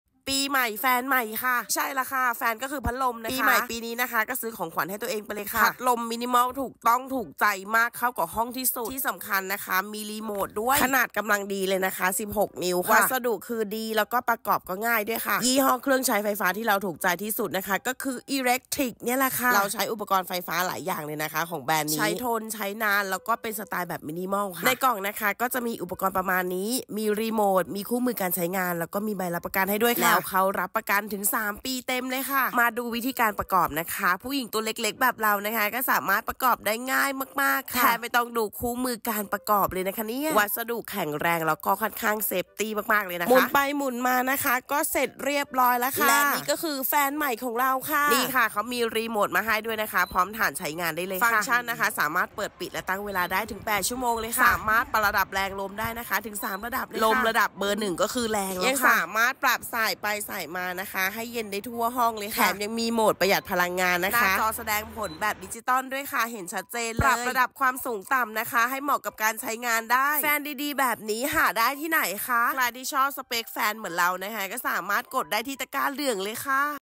ปีใหม่แฟนใหม่ อิอิ Fan = พัดลม สไตล์มินิมอล มีรีโมทใช้งานสะดวกสุดๆ ขนาด 16“
ปีใหม่แฟนใหม่ อิอิ Fan = พัดลม สไตล์มินิมอล มีรีโมทใช้งานสะดวกสุดๆ ขนาด 16“ กำลังดีเลย ลมแรงมาก รับประกันถึง 3 ปีใช่งานกันคุ้มๆ เลย